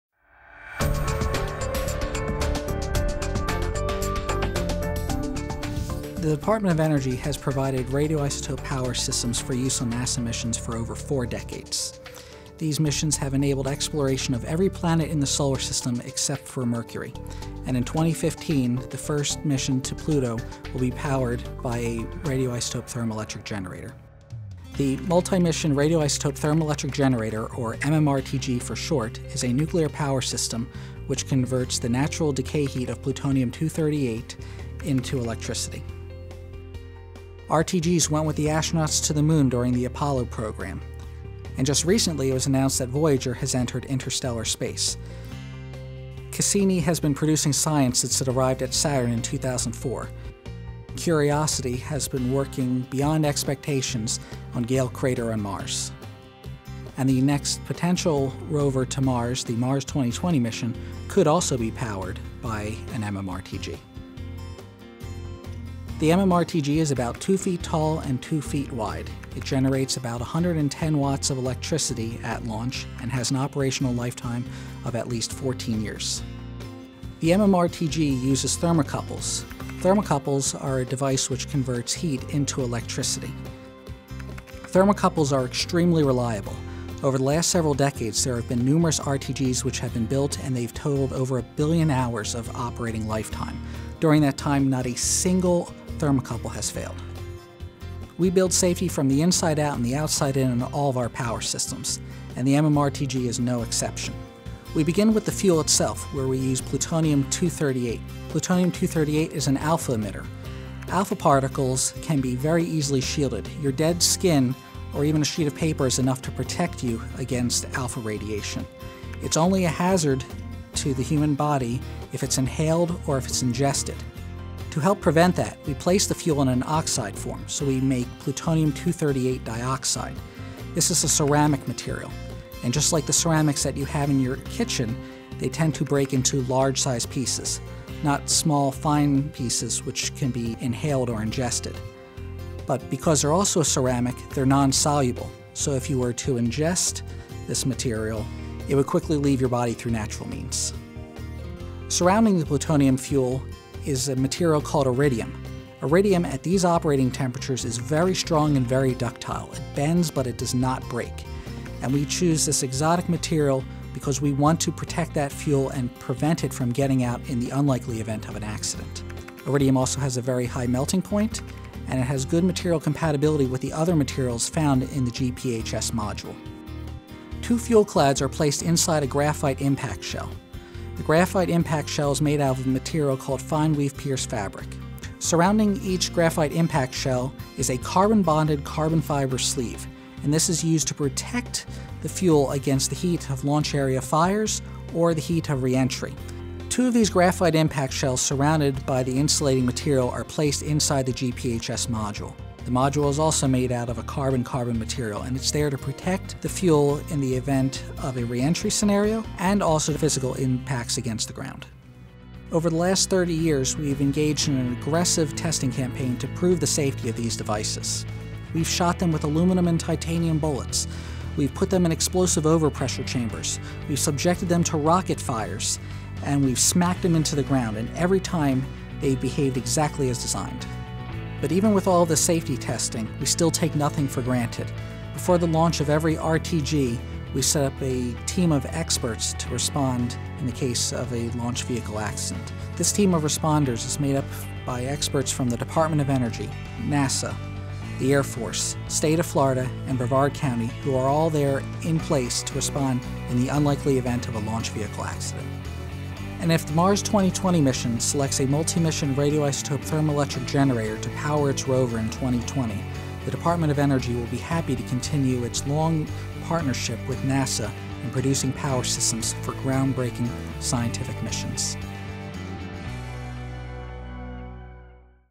Mars2020 MMRTG
Information about the nuclear power system that could be used by NASA's next potential Mars rover mission, now known as the Mars 2020 rover.